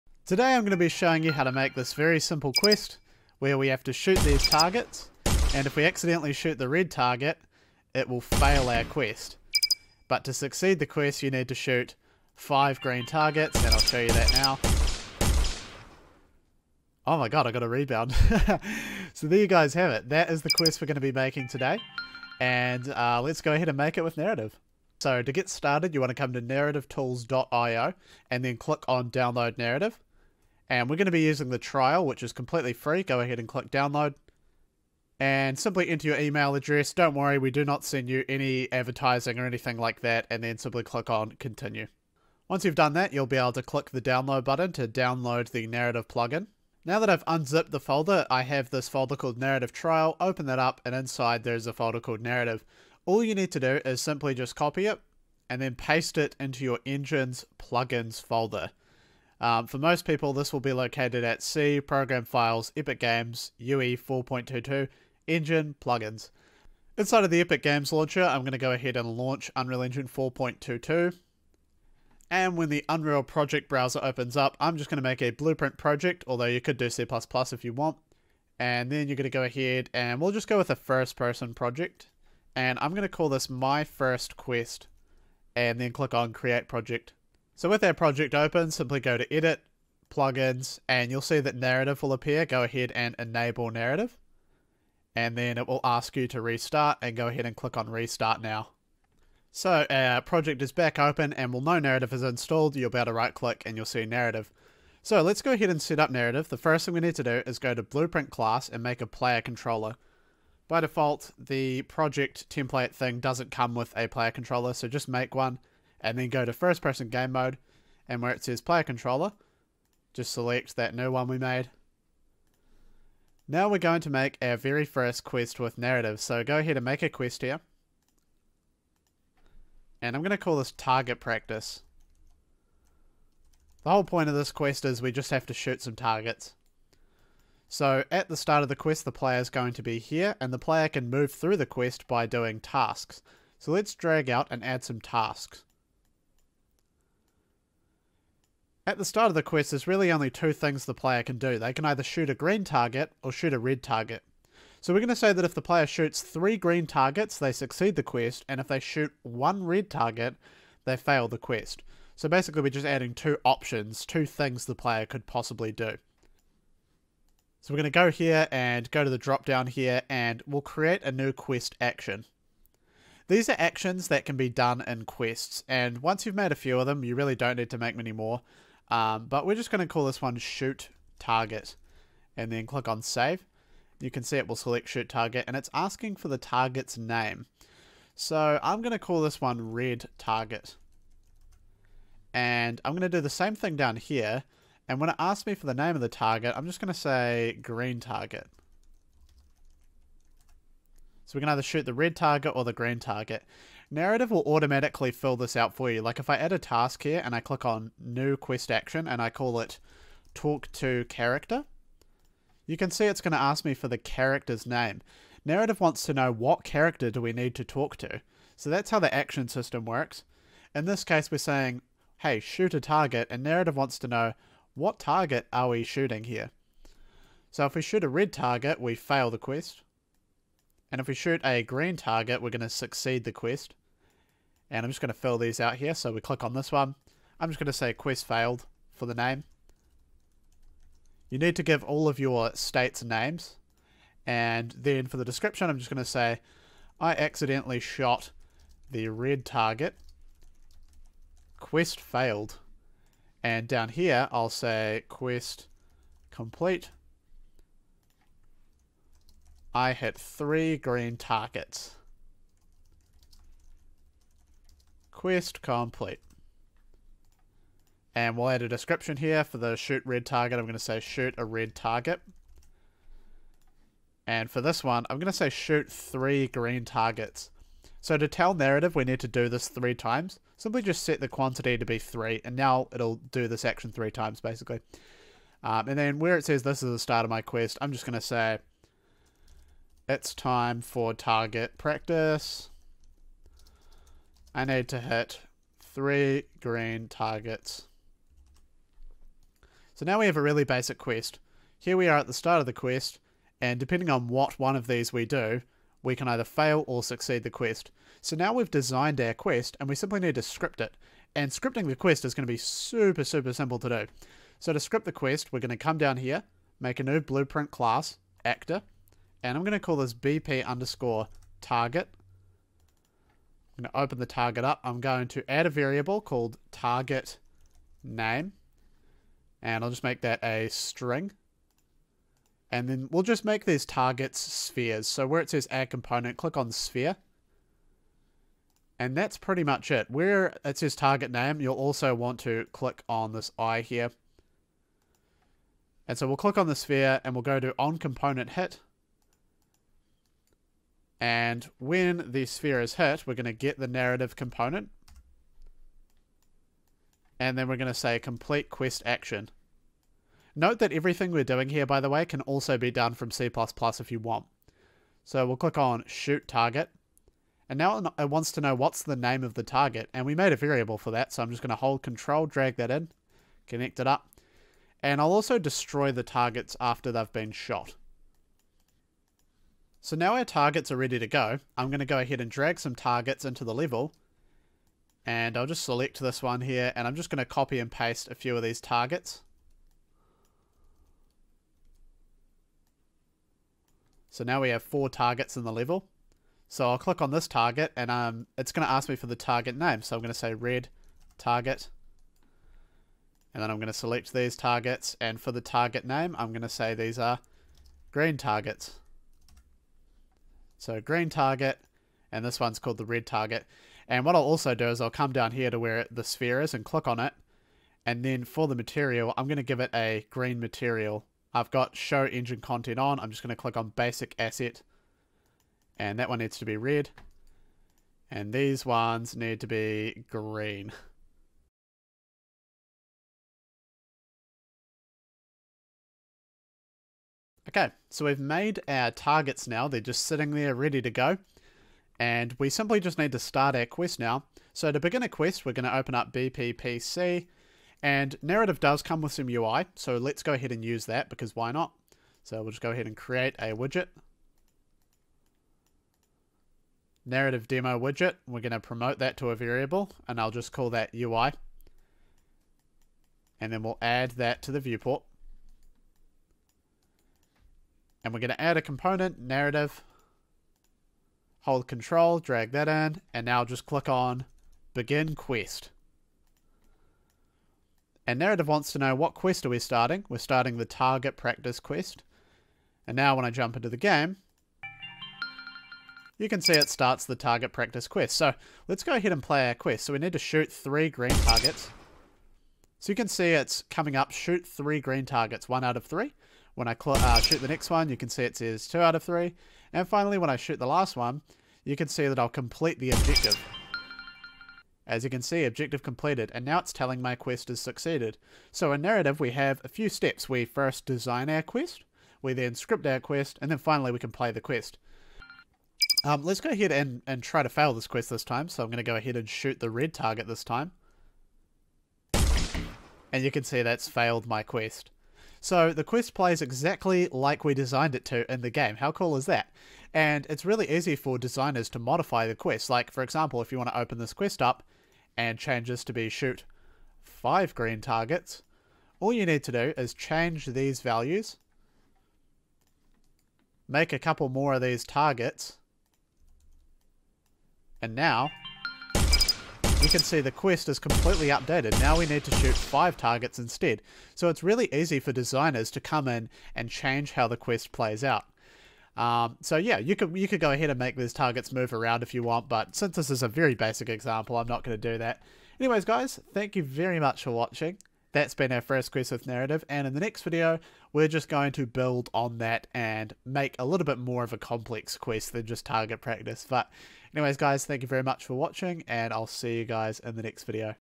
Narrative: Add Quests to your Game in 10 minutes
Narrative has been completely rewritten from the ground up and now has a lot more features, and much better performance.

Narrative can handle thousands of Quests, and quests can be non-linear, and have as many steps as required.

If you have already purchased the original version of Narrative, please email and I will send you the new version free of charge.